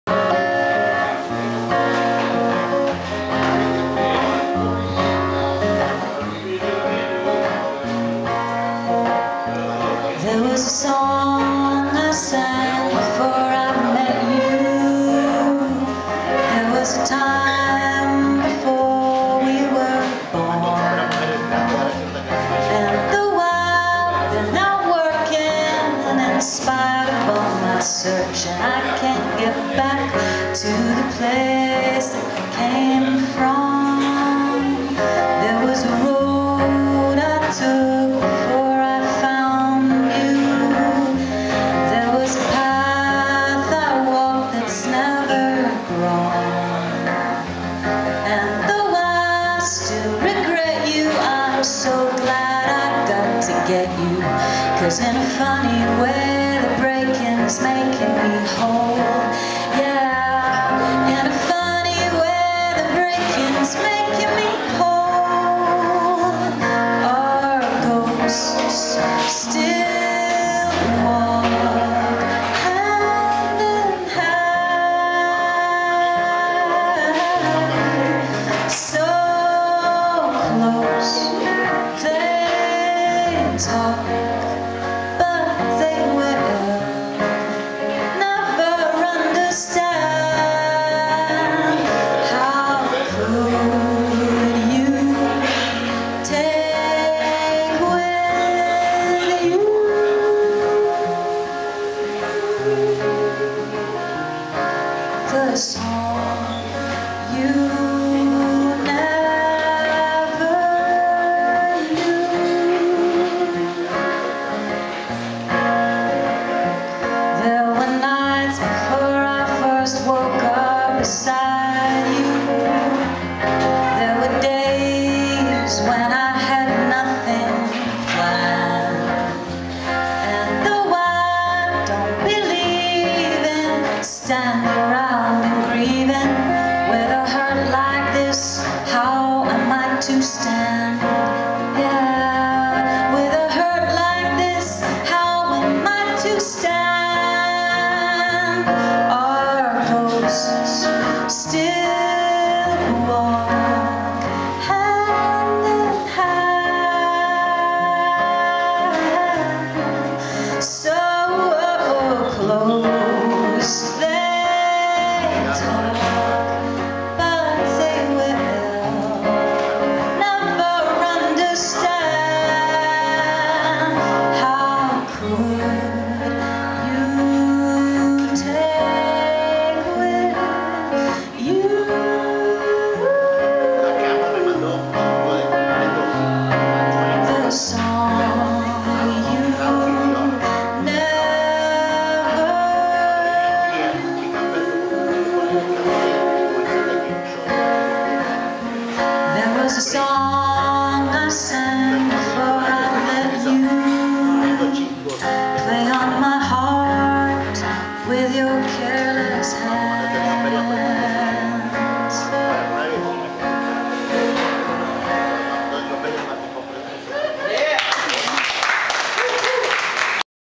Sylvie Lewis @ South By Soup Fest 2012
Sylvie Lewis from London took time to play South By Soup Fest for the clients at the Caritas of Austin's Community Kitchen during her recent visit to Austin.